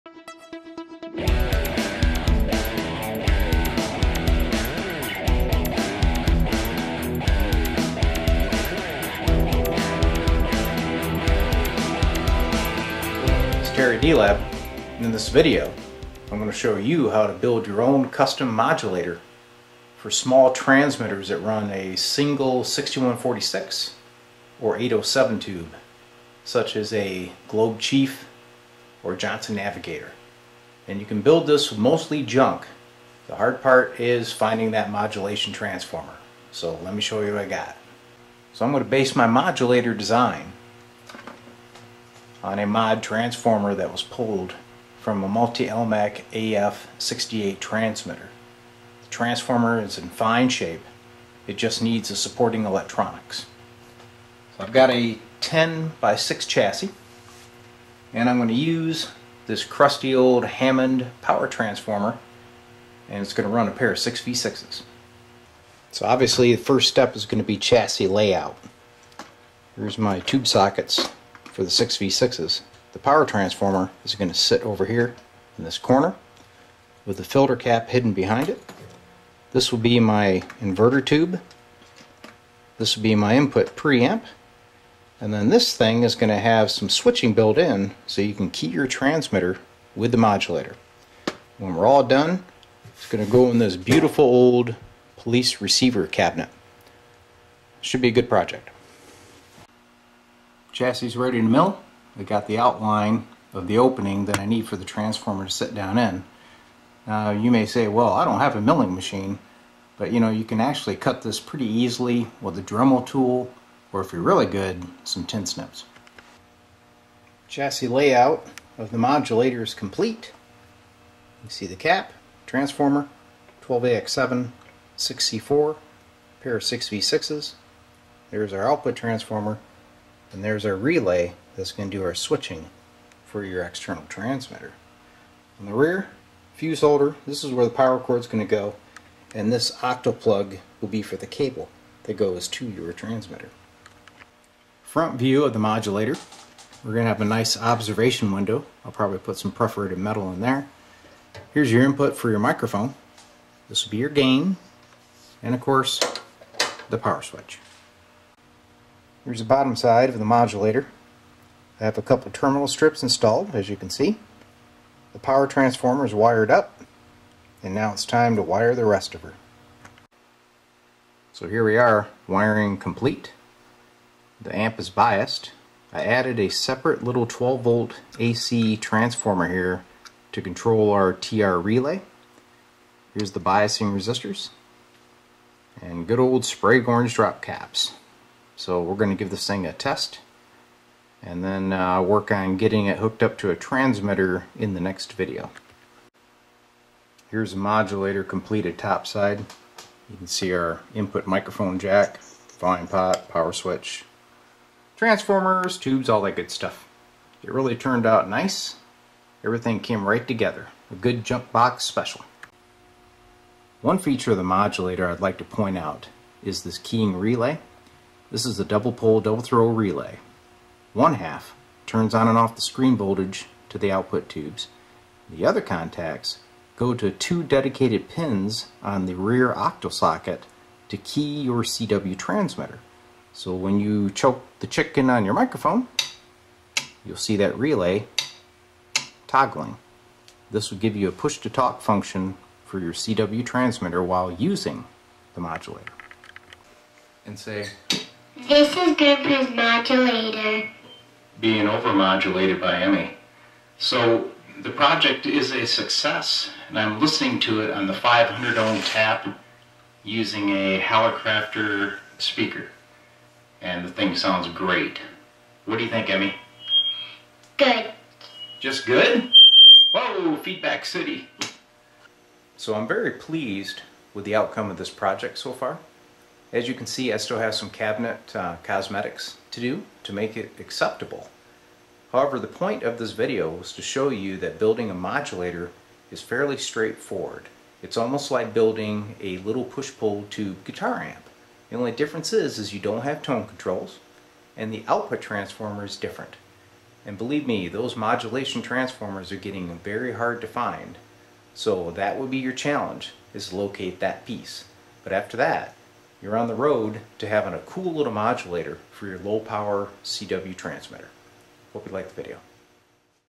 How to build a 6v6 tube ham transmitter AM modulator by d-lab electronics
A great way to utilize your junk box items and make a cool modulator for your CW rig. This is the 3rd one that I have made. This one uses a Multi-Elmac transformer.